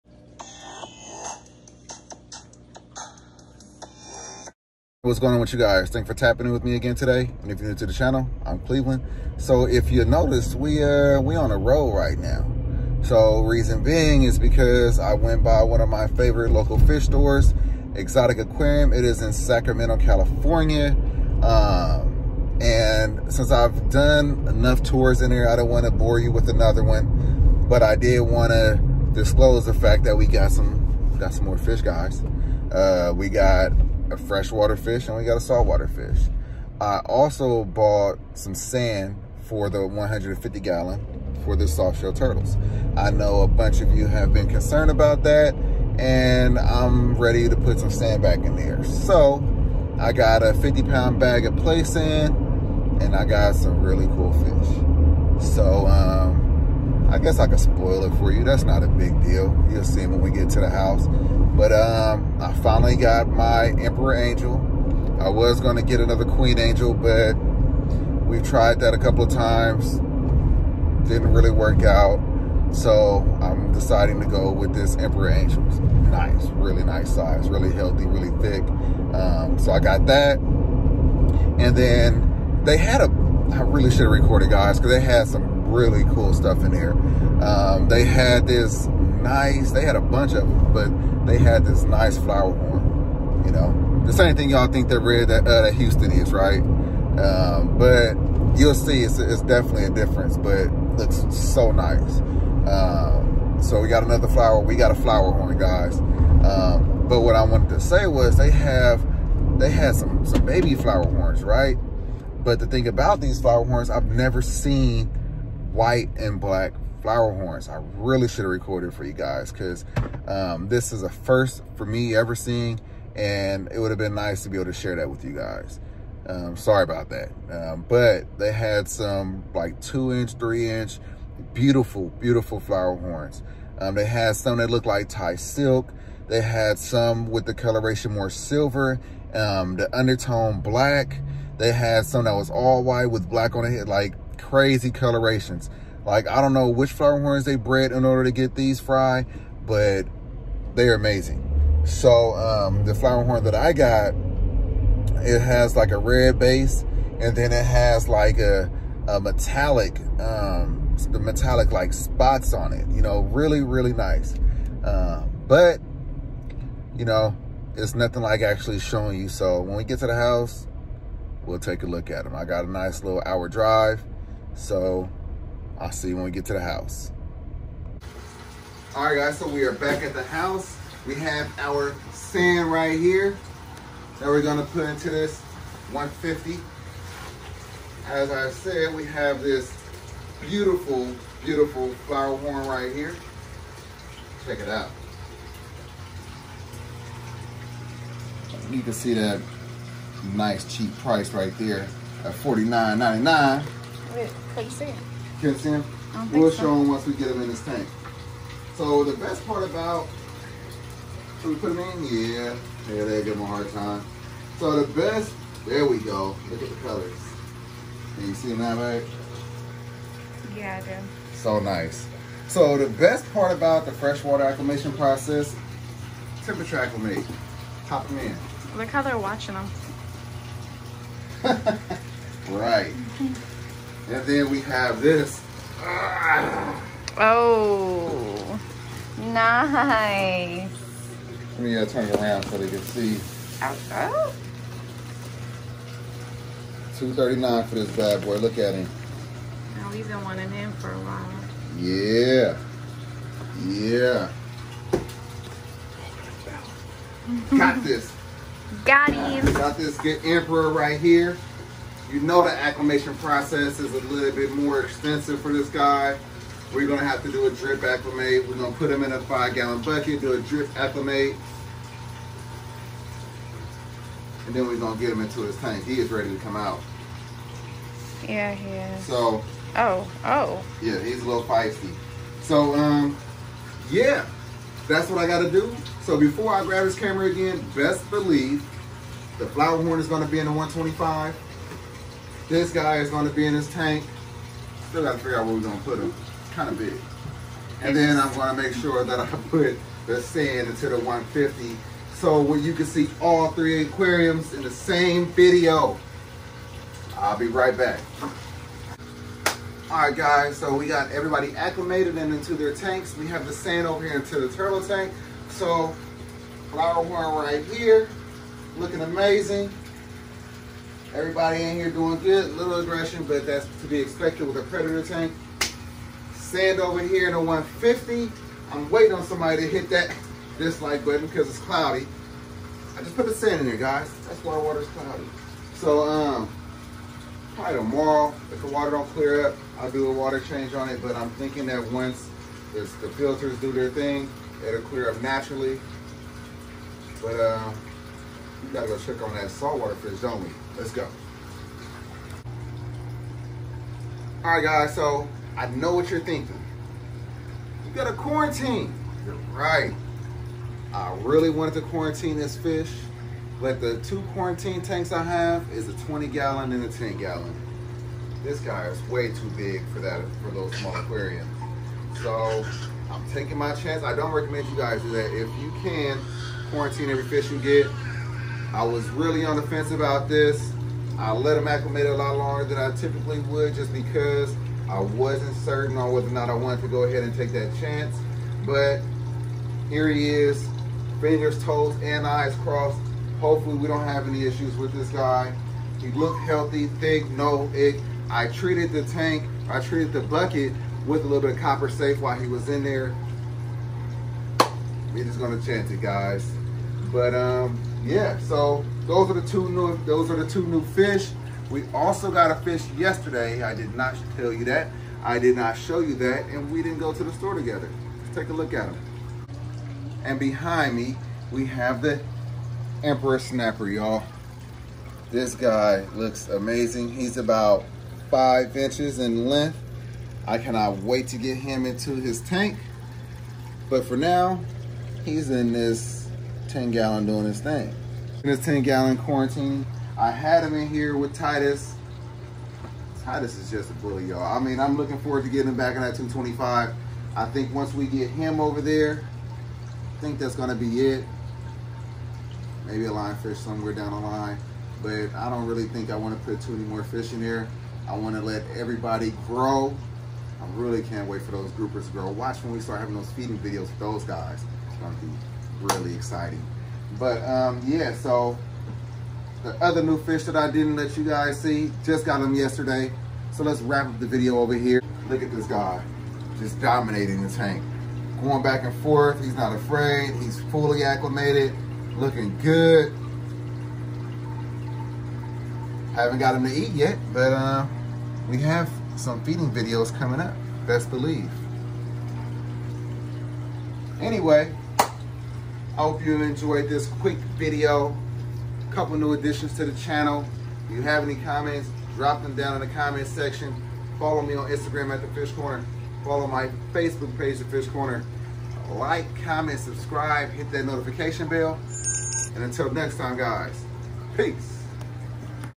NEW FISH!! *SALTWATER & FRESHWATER*
In this video I’m introducing you to a couple new additions to the channel. We also bought some sand for the Florida Soft Shells. I bought the fish and sand at Exotic Aquarium In Sacramento Ca, one of my favorite fish stores! If you haven’t stopped by there, you really should! I bought a flowerhorn and Emperor Angel! So excited about these two new additions, I’m not done though, we’re buying another Angel! There’s nothing like buying New Fish!